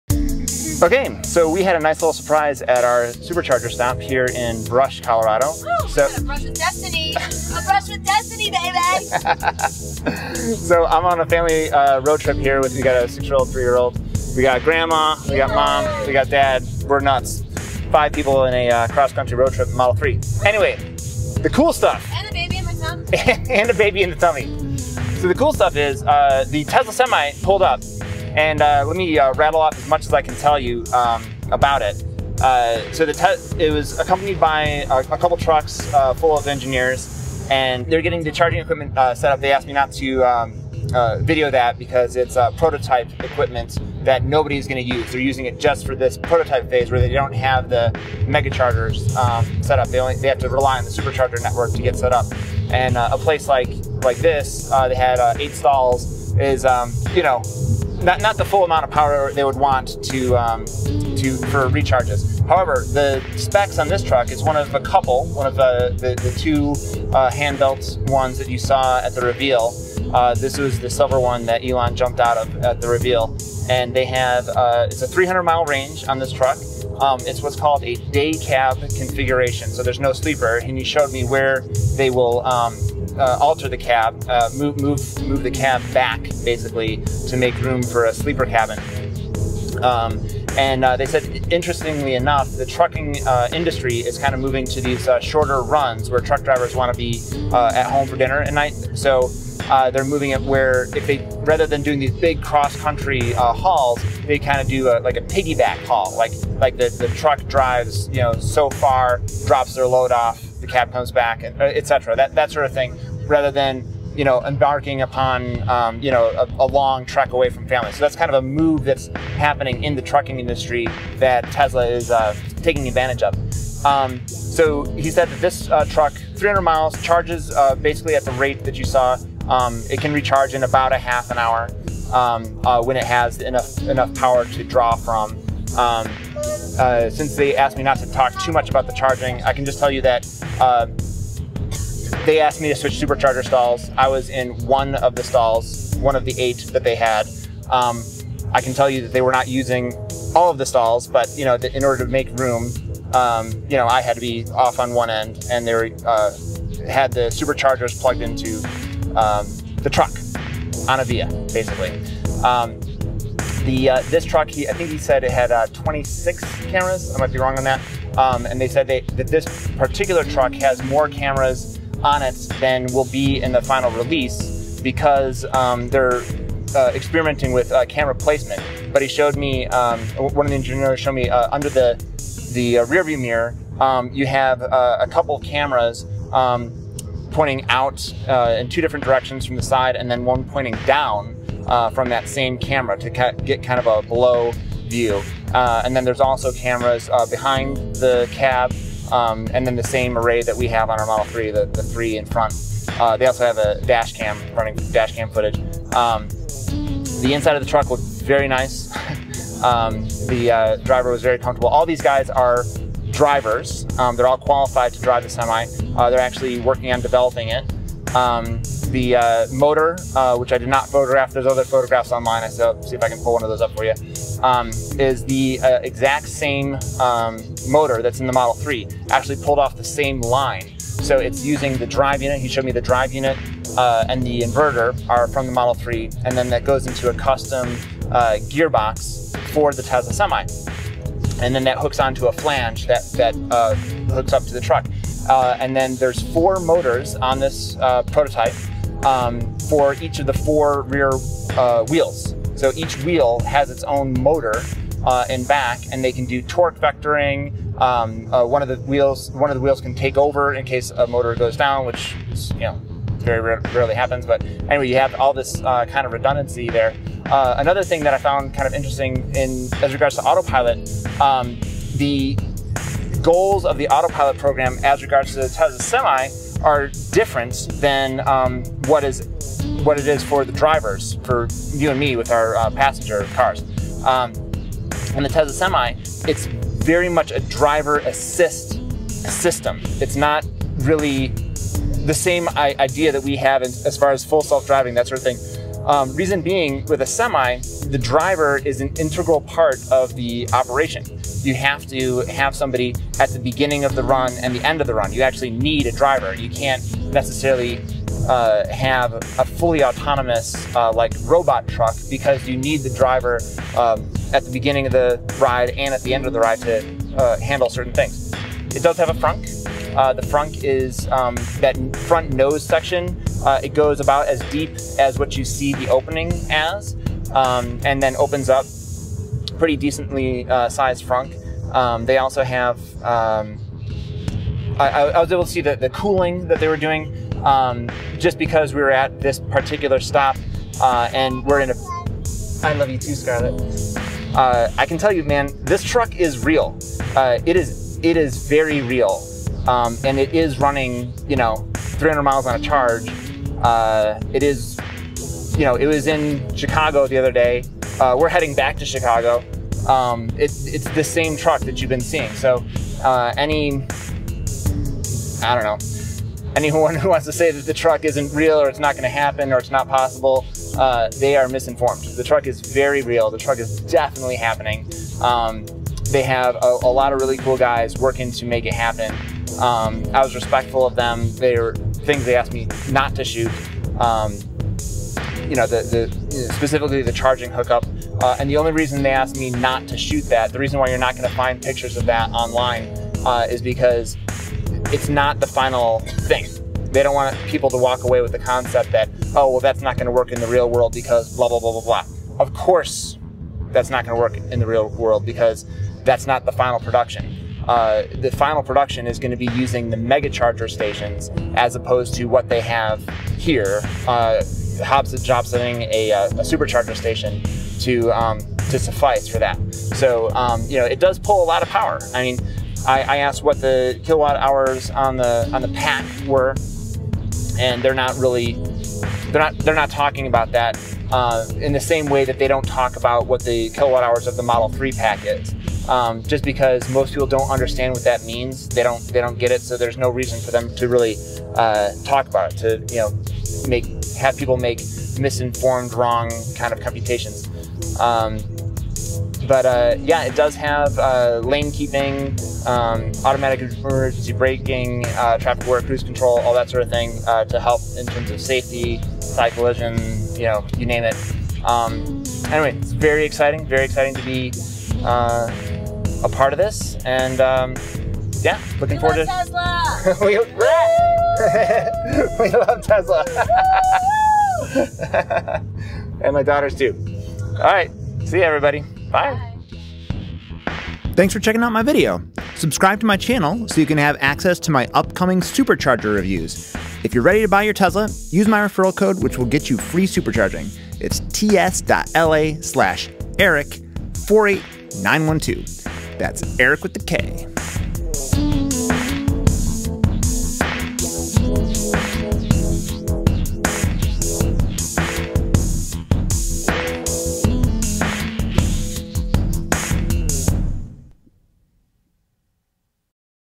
Tesla Semi - New Details
The Tesla Semi made a surprise appearance at one of our Supercharger stops! It was on its way across country when it stopped for a charge at the Brush Colorado Supercharger. Here are some of the key details the engineers told me:

This version is a "Day Cab" with a 300 mile range and it takes 30 min to recharge when it has adequate power. Currently it is using the Supercharger Network, but will utilize its own MegaCharger Network when it hits production. Tesla will be its own first customer and the initial truck sales will be to itself to relieve the financial burden in transporting materials from the Gigafactory.

This one-off Alpha prototype is made of carbon fiber. It has approximately 26 cameras around it for Autopilot, which will be a lower number once the truck design is final. This one has extra cameras so they can experiment with camera placement for optimal use in its driver assistance suite.